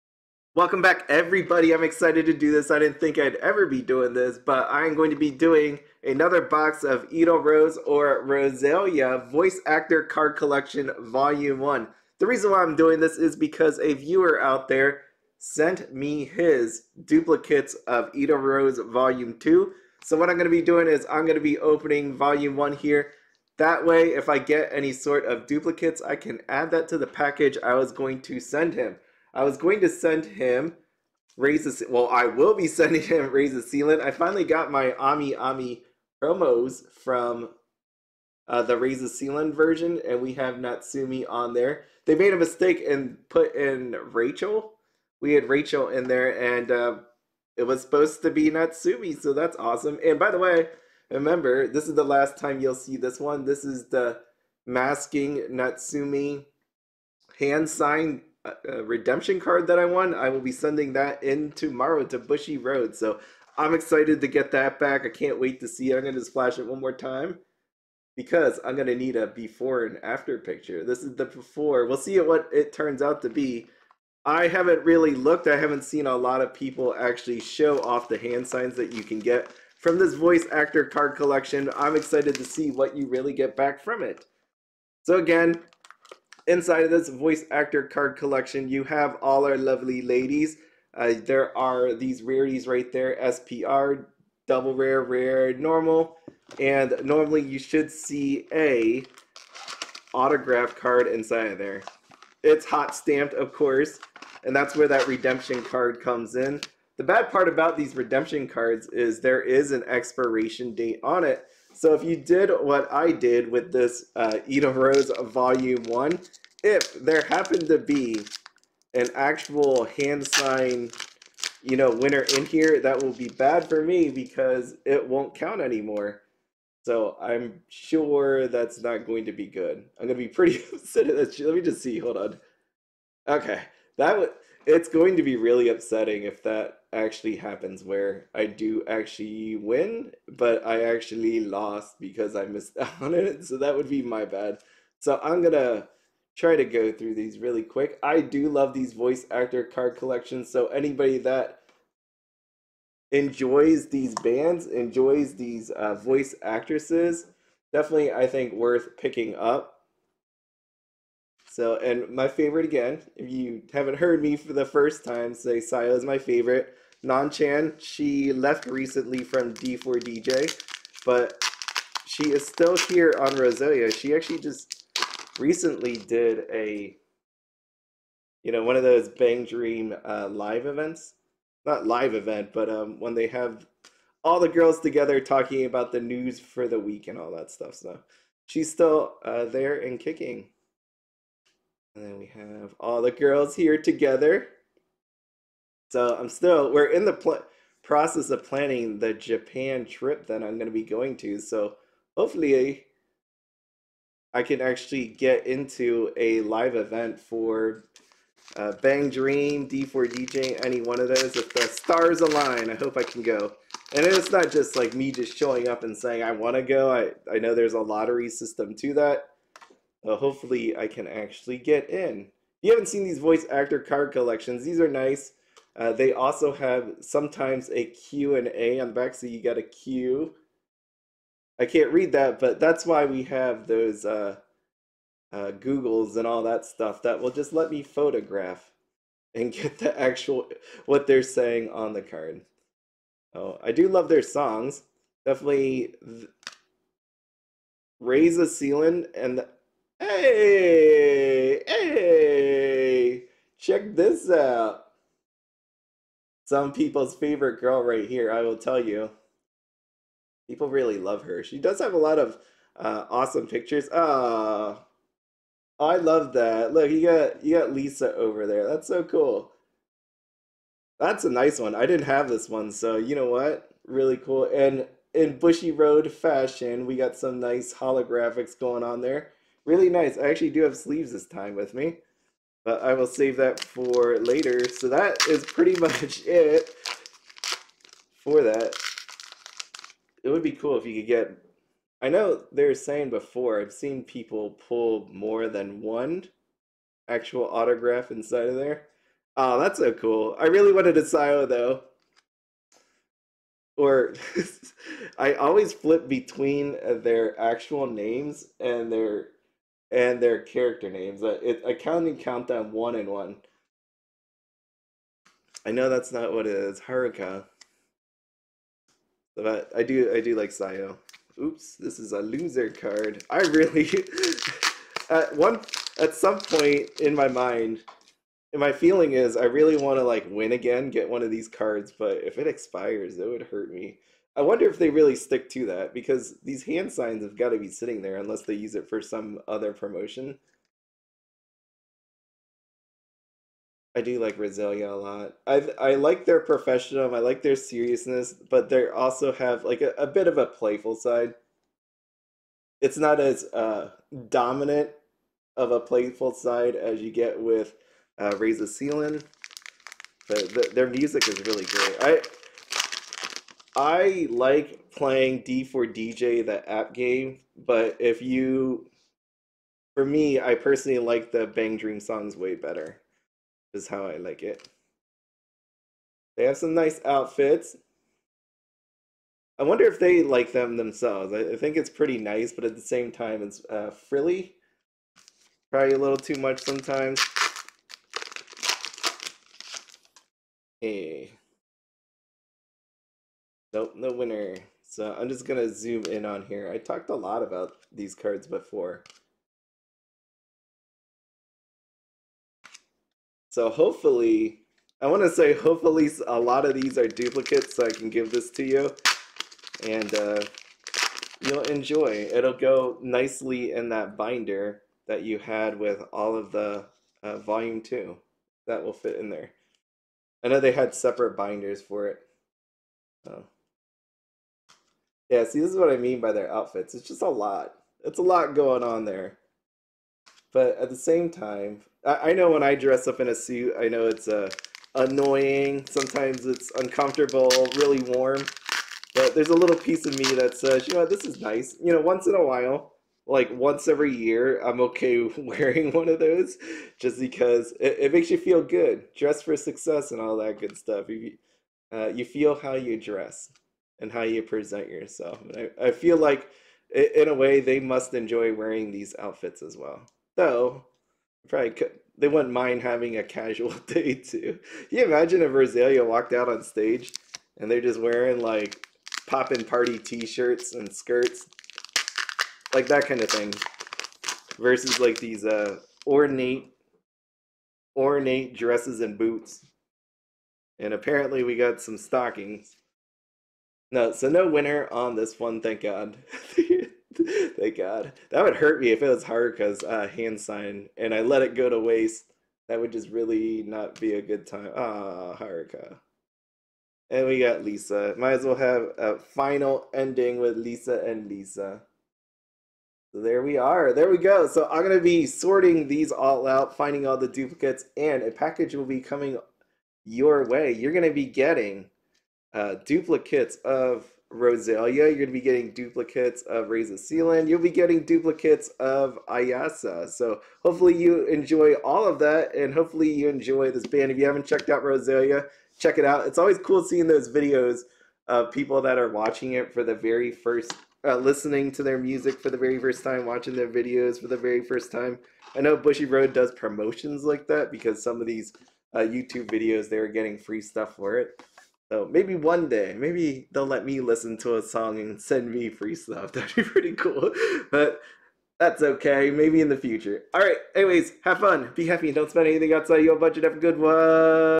Roselia (Edel Rose) Voice Actor Card Collection Set 1 Opening & Discussion
Today I open Roselia's / Edel Rose Voice Actor Card Collection Ex 01. I originally got this from the Bushiroad Global Online Store.

There are sign cards for
◆ Aina Aiba as Yukina Minato ◆
◆ Haruka Kudo as Sayo Hikawa ◆
◆ Yuki Nakashima as Lisa Imai ◆
◆ Megu Sakuragawa as Ako Udagawa ◆
◆ Kanon Shizaki as Rinko Shirokane ◆